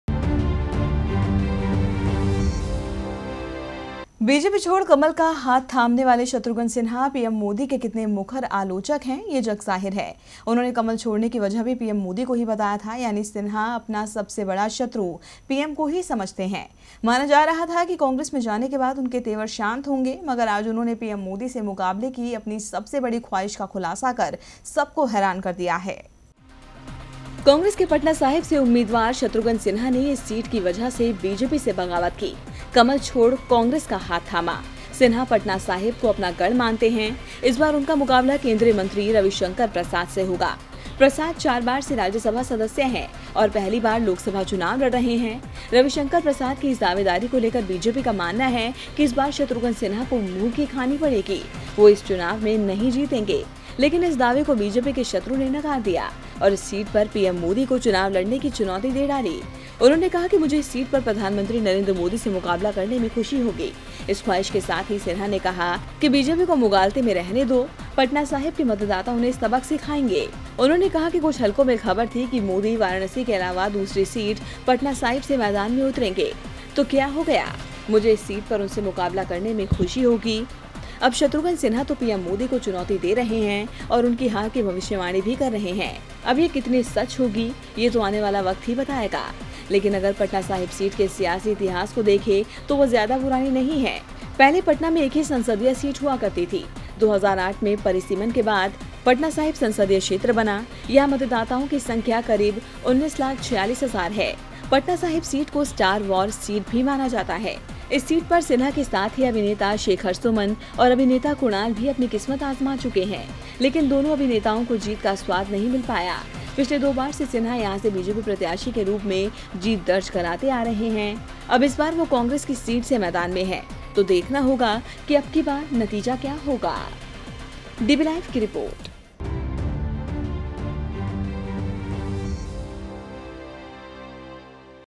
शत्रुघ्न सिन्हा ने बताई अपनी ख्वाहिश |अबकी बार होगा मोदी बनाम Shatrughan Sinha !
बीजेपी छोड़ कांग्रेस का हाथ थामने वाले शत्रुघ्न सिन्हा पीएम मोदी के कितने मुखर आलोचक हैं, ये जगजाहिर है...उन्होंने कमल छोड़ने की वजह भी पीएम मोदी को ही बताया था...यानी सिन्हा अपना सबसे बड़ा शत्रु पीएम को ही समझते हैं... माना जा रहा था कि...कांग्रेस में जाने के बाद उनके तेवर शांत होंगे...मगर आज उन्होंने पीएम मोदी से मुकाबले की अपनी सबसे बड़ी ख्वाहिश का खुलासा कर सबको हैरान कर दिया...